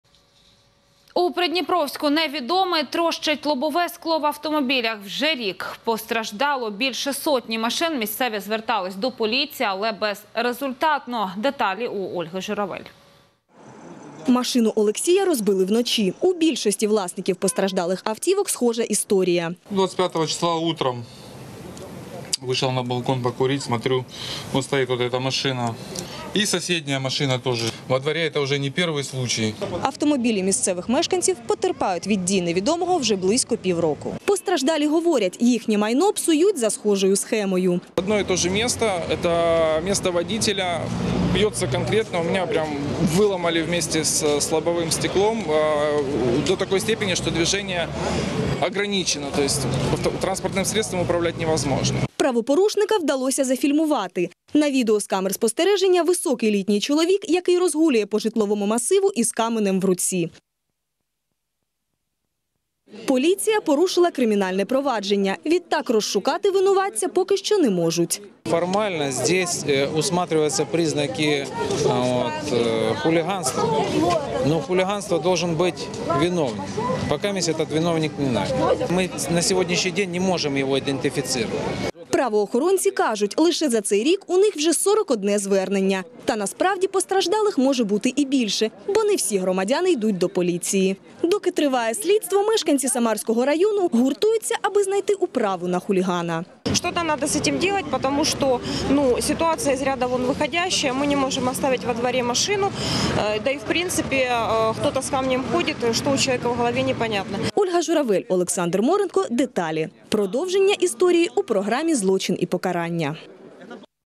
В Приднепровске хулиган уже полугода бьет машины: что говорит полиция?
В Приднепровской неизвестный крушит лобовое стекло в автомобилях. Уже больше, чем полгода. Пострадали более сотни машин. Местные обращались в полицию. Но безрезультатно.  В полицию за подробностями обратились и журналисты Деталей на 34 канале.